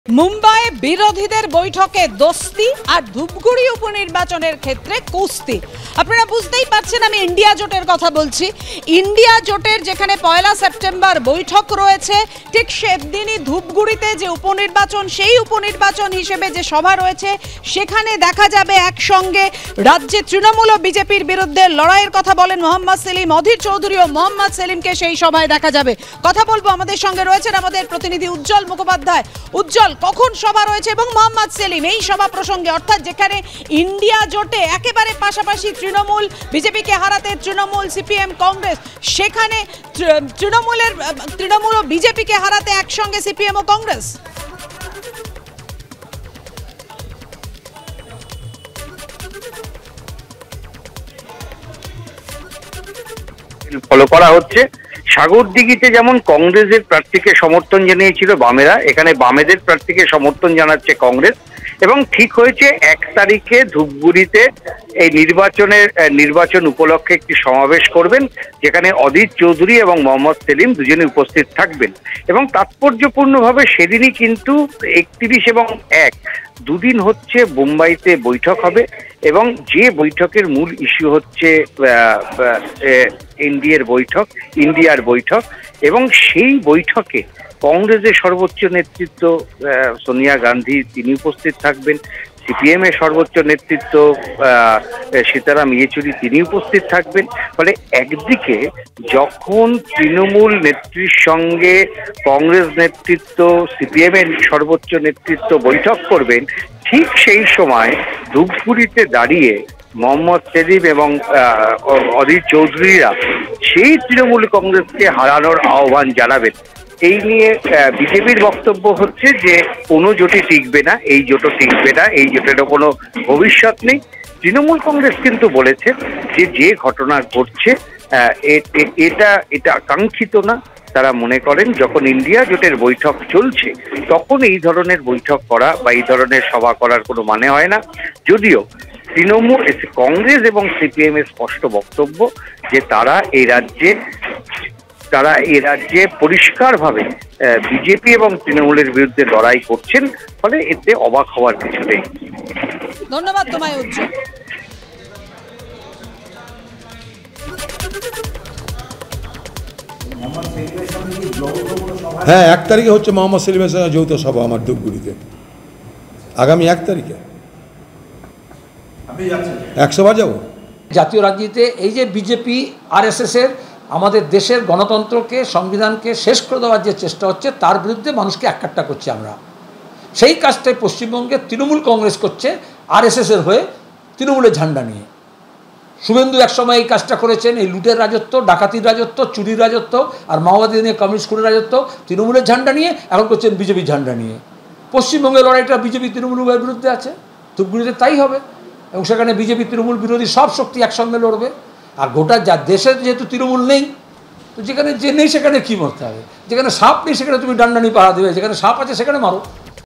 CPM-Congress : মুম্বইতে দোস্তি, আর ধূপগুড়িতে দোস্তি, তৃণমূল বিরোধী প্রচারমঞ্চে একসঙ্গে অধীর-সেলিম
CPM-Congress : মুম্বইয়ে বিরোধী বৈঠকে 'দোস্তি' আর ধূপগুড়ি উপনির্বাচনে 'কুস্তি'! ১ সেপ্টেম্বর মুম্বইয়ে ইন্ডিয়ার বৈঠকের দিনই বাংলায় তৃণমূলের (TMC) বিরোধিতায় একজোট হতে চলেছেন অধীর রঞ্জন চৌধুরী (Adhir Ranjan Choudhury) ও মহম্মদ সেলিম (Md Selim)। প্রসঙ্গত, ধূপগুড়ি উপনির্বাচনে (Dhupguri By Election) তৃণমূলের বিরুদ্ধে একজোট সিপিএম-কংগ্রেস। ১ সেপ্টেম্বর ধূপগুড়িতে বাম প্রার্থীর সমর্থনে একমঞ্চে- প্রচার করবেন অধীর চৌধুরী ও মহম্মদ সেলিম। রাজ্যে তৃণমূল ও বিজেপির (BJP) বিরুদ্ধে লড়াই চলবে, প্রতিক্রিয়া মহম্মদ সেলিমের।